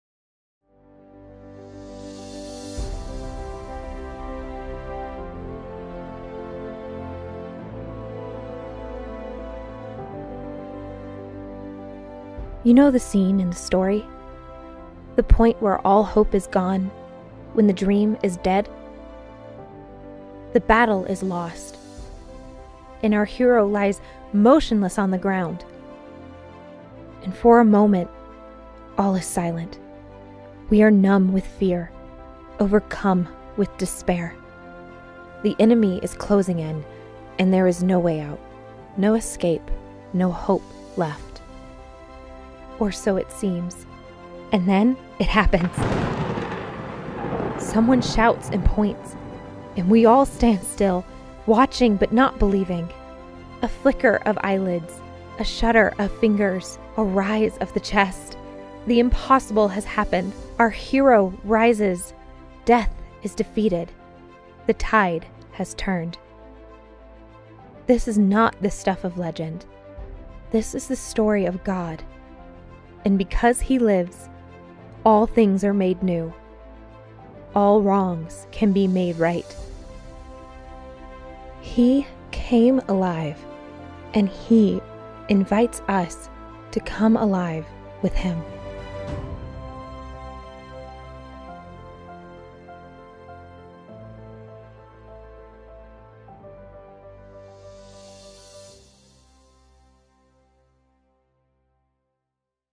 Easter Sunday
Got plans Sunday? You do now! Join us for a special series called Come Alive: A Journey Through Holy Week at Prayer House Ag - 1525 24th Ave Kenosha, WI 53140. Sunday @10am & Sunday evening @ 5pm for kids/Youth